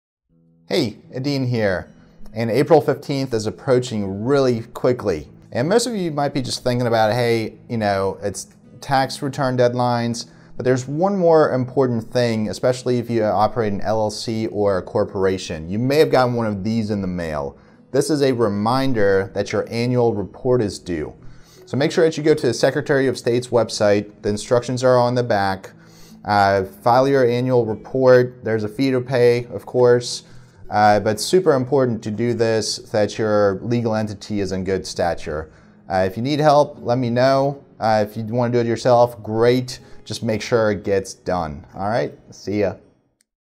Annual Reports for LLC and Corporations
In North Carolina, all LLCs and Corporations must file annual reports with the Secretary of State... by April 15th!!